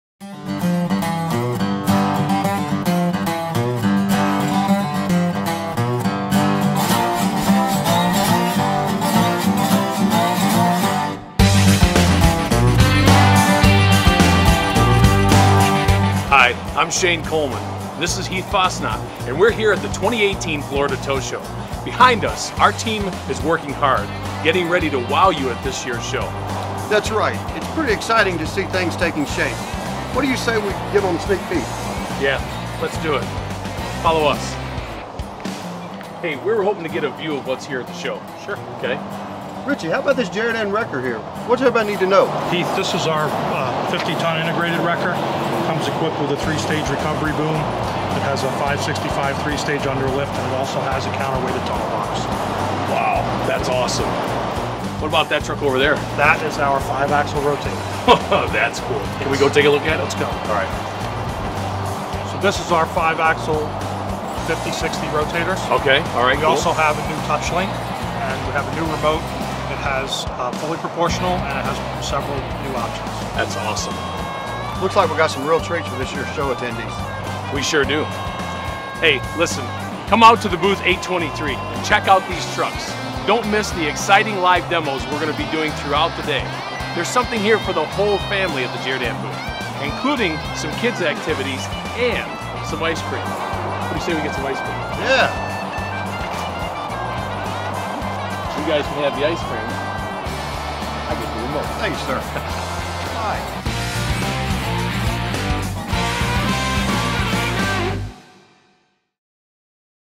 The Florida Tow Show continues with Jerr-Dan Pride!
This year’s FloridaTowShow will certainly be fun-filled, something that the whole family can enjoy. We have product demos as well as all your favorite Jerr-Dan carriers, wreckers and rotators for you to check out. Stop by booth #823 and join the JerrDanPride team.